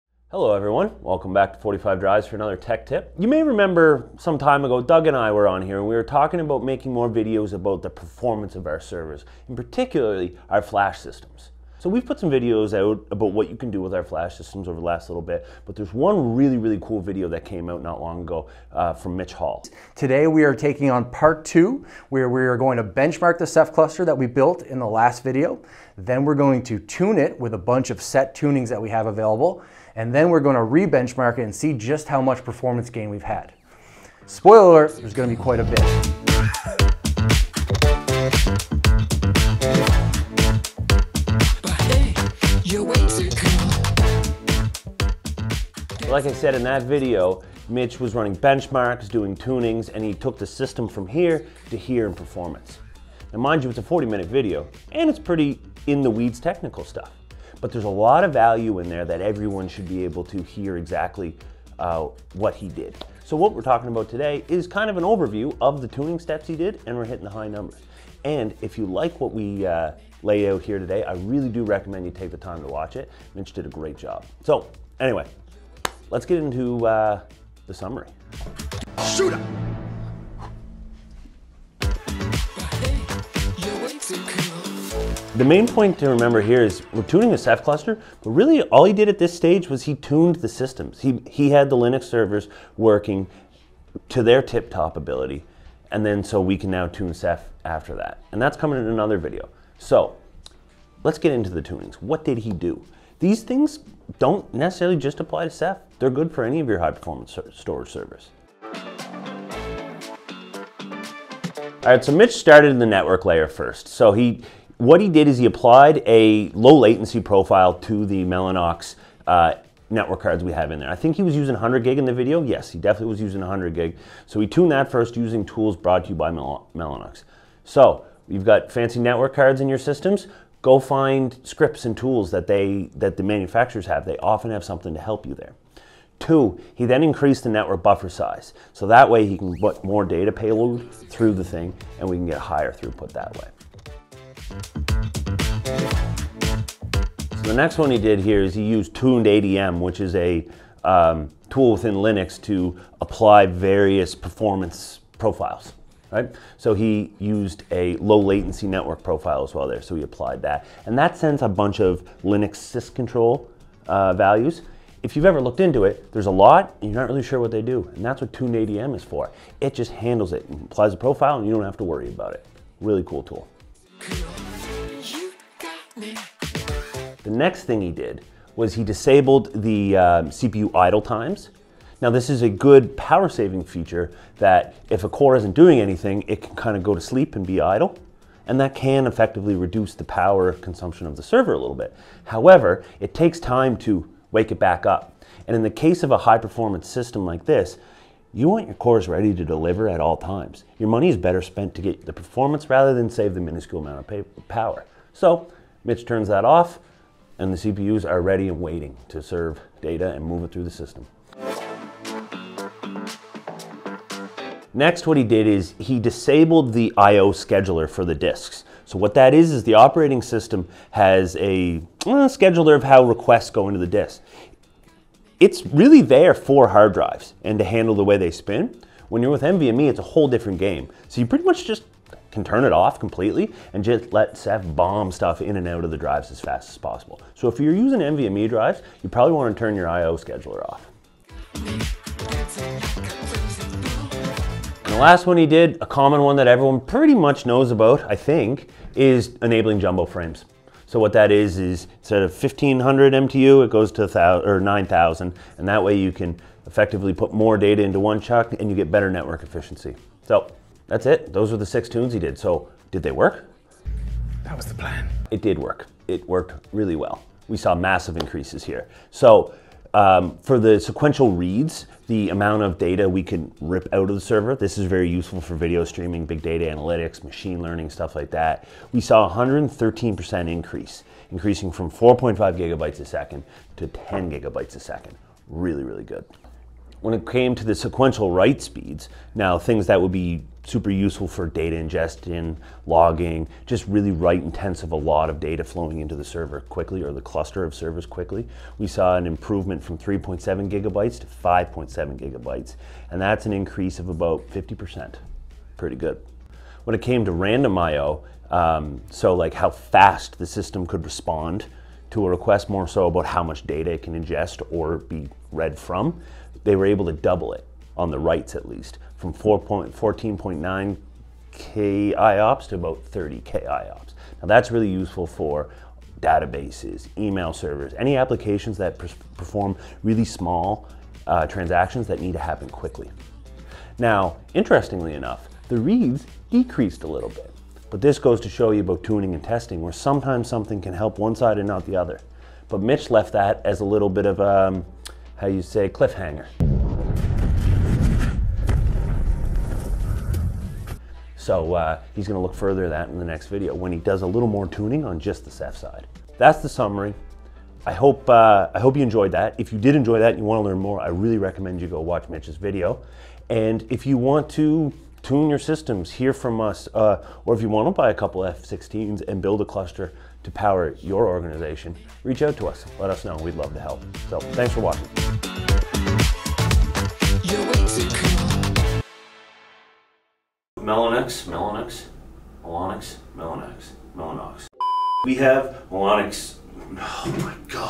Ceph NVMe Cluster: 6 Key Performance Tweaks You Need to Know!
Unlock MAX Ceph NVMe Performance – In Minutes!

If you’ve ever wondered how to truly push your Ceph NVMe cluster to its limits, this is your shortcut. In this video, we break down 6 game-changing performance tweaks—originally showcased by Mitch Hall—that delivered up to 113% faster read speeds, 50% faster writes, and doubled IOPS.
Whether you’re working with Ceph storage, NVMe drives, or any high-performance Linux server, these tuning steps can help you squeeze every last drop of power from your hardware.

What You’ll Learn in This Summary
We’ve condensed a 40-minute deep dive into an easy-to-follow performance blueprint:
Network Layer Optimization – Applying low-latency profiles to Mellanox network cards and increasing network buffer size for higher throughput.
Tuned ADM Profiles – Using Linux’s built-in performance profiles to quickly set optimal sysctl values.
Disabling CPU Idle States – Keeping your CPU cores awake and ready for maximum responsiveness.
Turning Off the IO Scheduler – Unlocking raw NVMe potential by bypassing unnecessary scheduling.
Enabling Jumbo Frames (9000 MTU) – Improving network efficiency for large data transfers.
Hardware-Specific Manufacturer Tools – Leveraging vendor-provided utilities for extra fine-tuning.

Real-World Performance Gains
These tweaks aren’t just theory—they’ve been tested in the lab:
 • Sequential Reads: 4.5GB/s  10GB/s (+113%)
 • Sequential Writes: 3.7GB/s 5.7GB/s (+50%)
 • Random Write IOPS: 14.9K  30K (2× faster)
Perfect for workloads like video streaming, big data analytics, machine learning, databases, and email servers.

In today’s data-driven world, speed equals productivity. The difference between a tuned system and a stock setup can mean faster research, smoother video playback, quicker analytics results, and an all-around better experience for your team and users.

Follow 45Drives for More Tips & Tech Demos

We post server optimization tips, Ceph tutorials, and hardware deep dives every week.